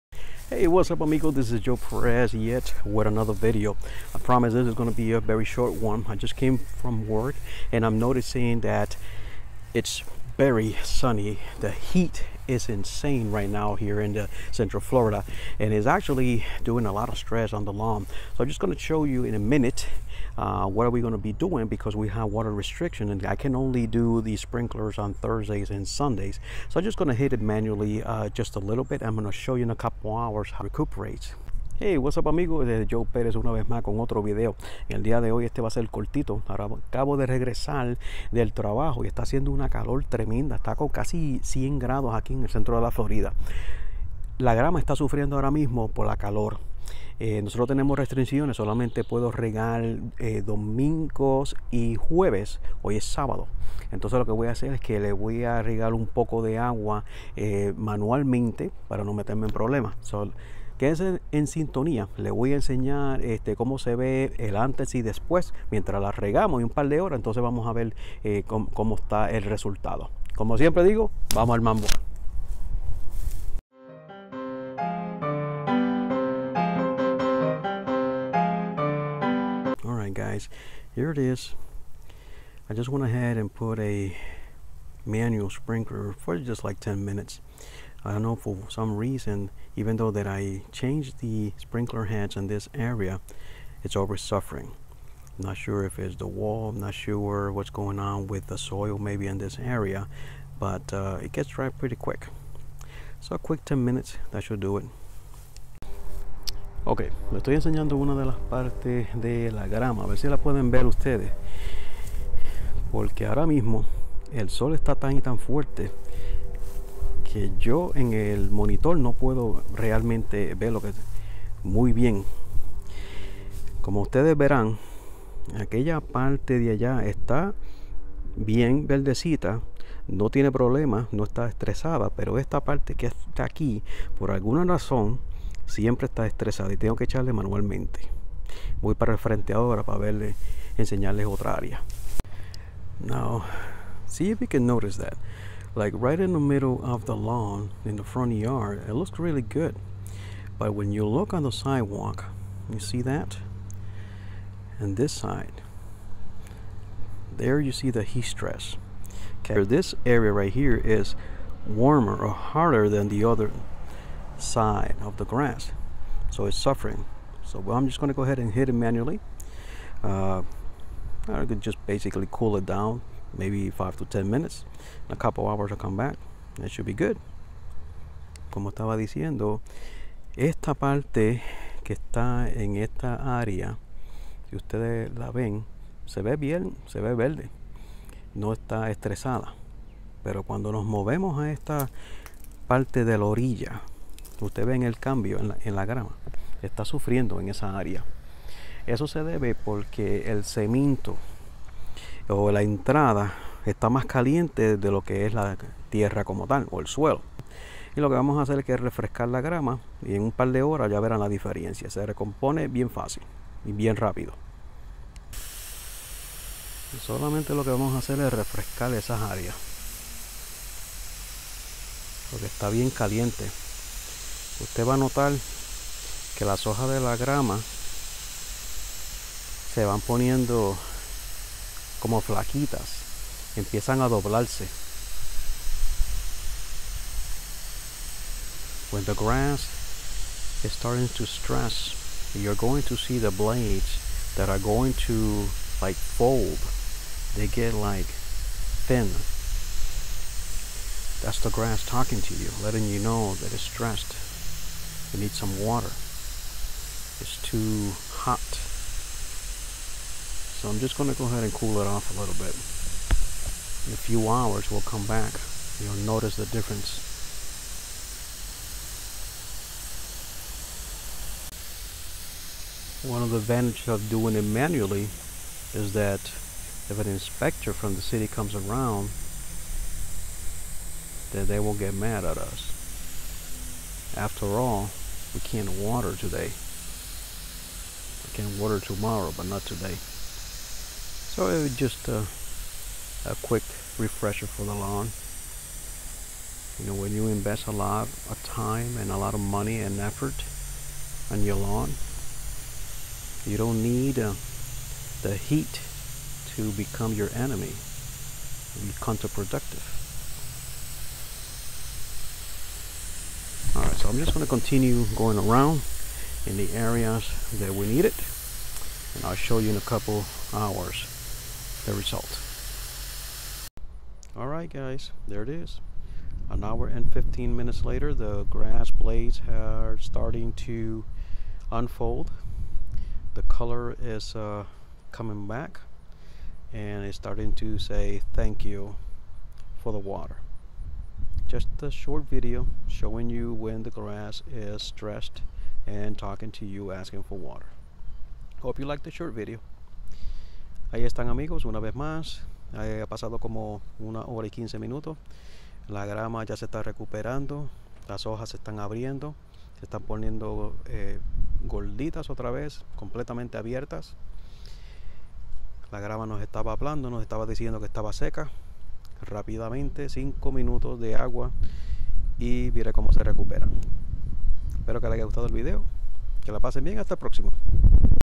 Lawn heat stress, Grama Pasto/ césped impactado por el calor
Hey what’s up Amigos Joe Perez here sharing a quick video about heat stress in my lawn. Just A quick solution to it. Your lawn will be grateful to you in just merely an  hour later.

Hey Whats up Amigos Joe Perez con  un video corto mostrandole el estress de mi grama/ pasto por motivos del calor.  FOrma rapIda y sencilla que su grama/pasta le agradecera apenas en 1 hora.

Joe Perez channel link